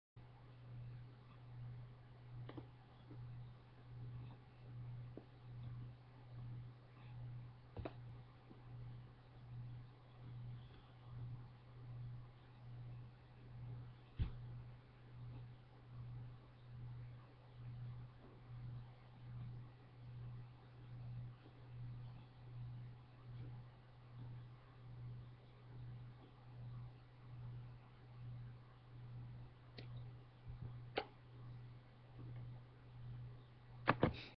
I has gadges ^^
i have gotten tiger eye stone plugs a little while ago .. and streched mah ears wif tapppers. if you wanna know haw i did it just request it and ima do it. okie, bai :)